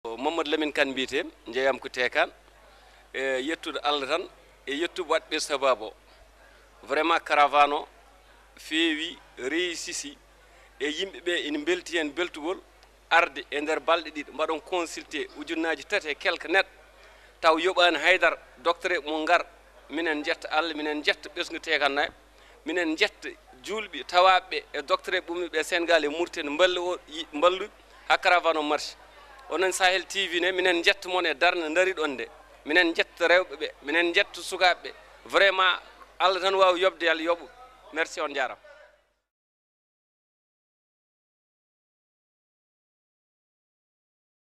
Caravane Santé ACTUME 2016 - Témoignage - Mamadou Lamine KANE, Bénévoletemoignage
Après la conférence de presse sur la caravane de santé 2016, en Mauritanie M. Mamadou Lamine KANE donne son impresssion sur la caravane de santé à Tékane et à Maghama.  En savoir plus